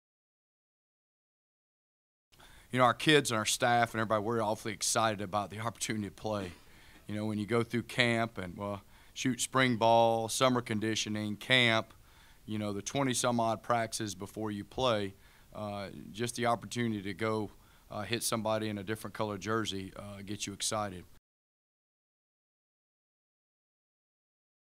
Kennesaw State Football Press Conference -- Week 1 vs. Samford
Head coach Brian Bohannon, along with junior QB Chandler Burks and senior LB Izzy Sam met with the media Monday afternoon for the Owls' first weekly press conference of the season.